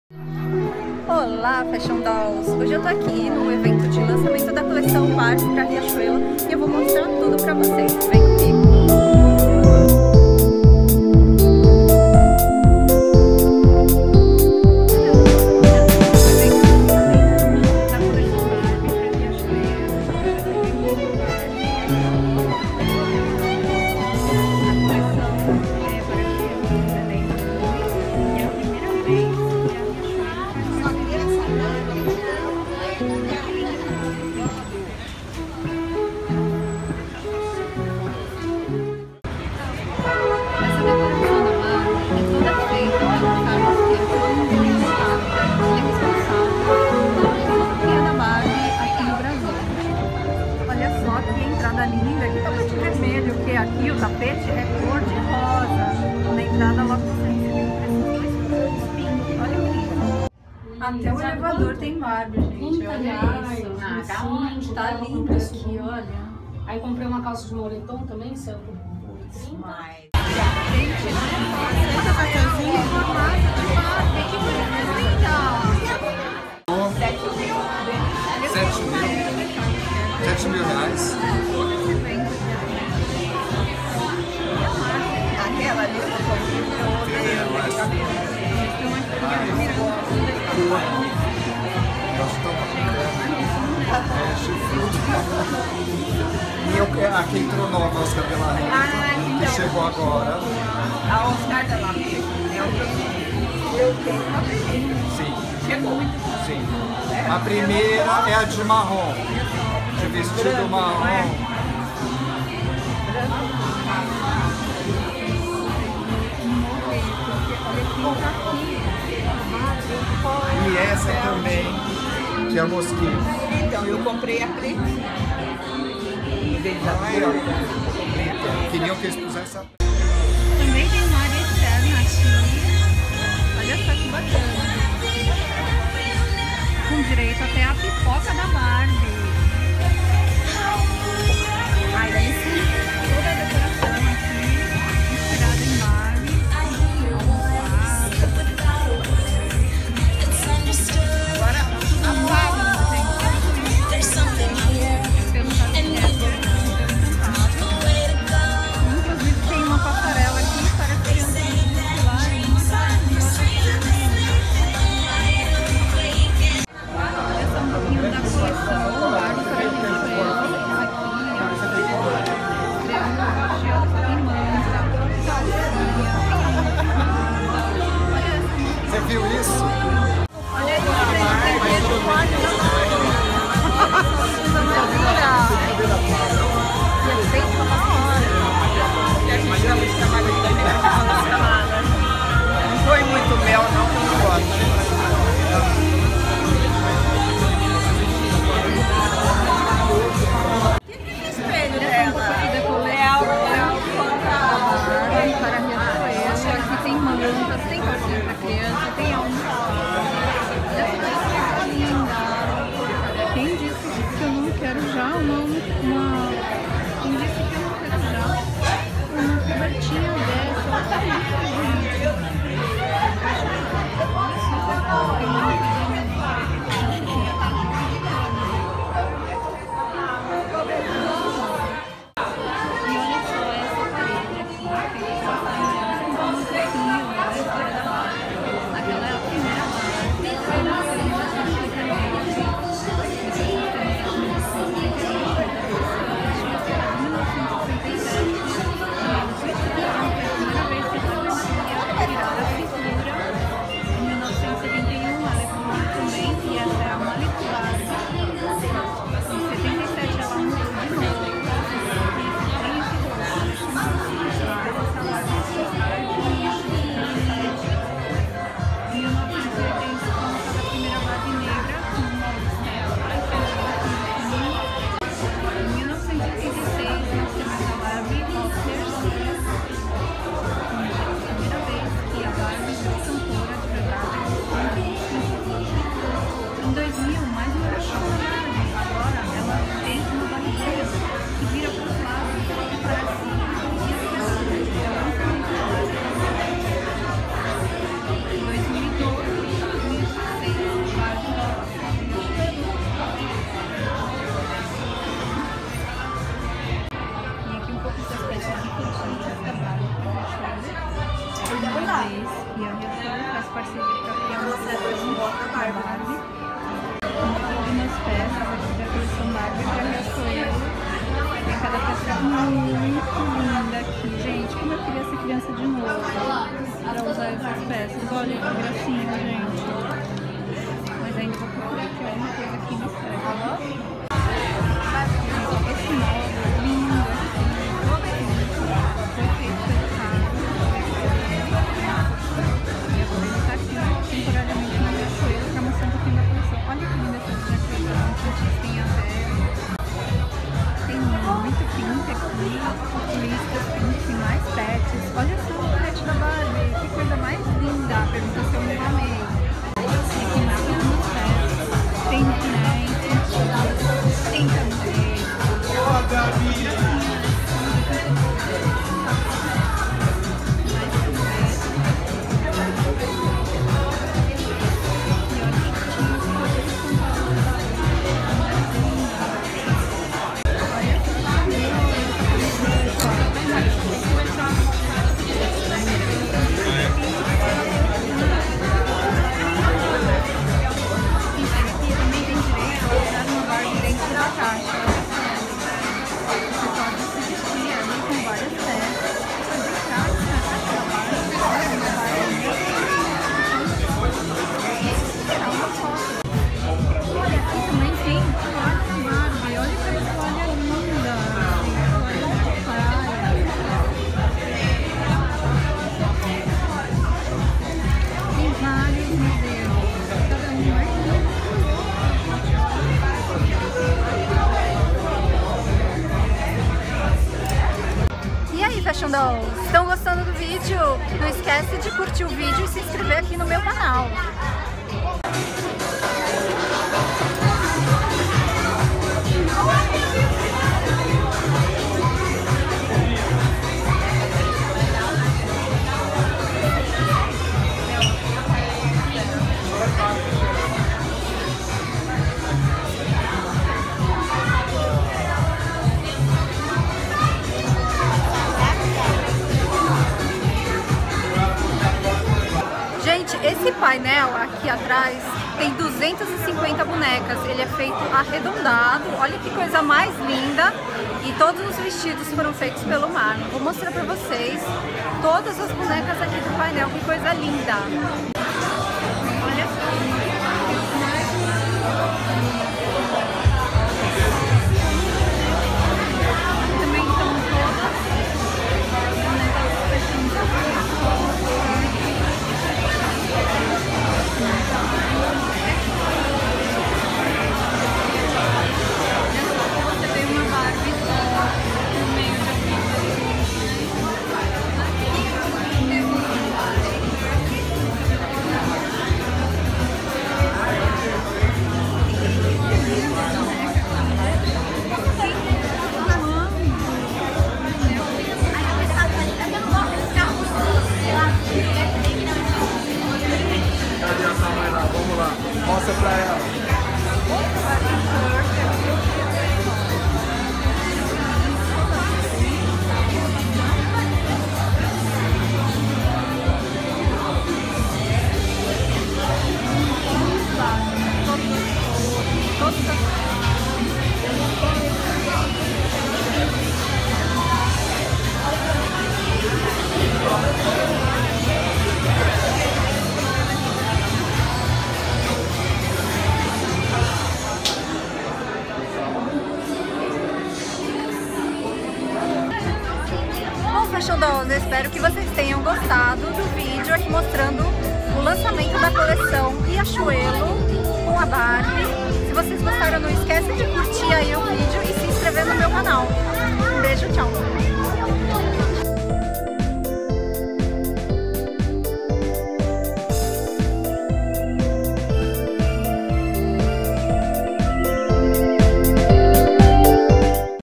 VLOG: Barbie Loves RCHLO | Patty Lye
Olá Fashion Dolls, nesse vídeo mostro tudo sobre o lançamento Barbie Loves RCHLO!
Passa lá no blog também pra conferir mais fotos do evento.
Snapchat: Patty_Lye (me segue lá pa ver coisinhas todos os dias)

beijos até o próximo vídeo!
Patty Lye.